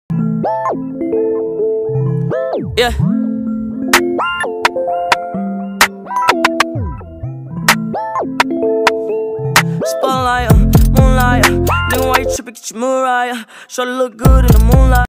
xxx Tentacion draw a card toon pictures
xxx Tentacion draw than he most professional Singer draw with Dom's pencil calur ## xxx Tentacion #xxxTentacion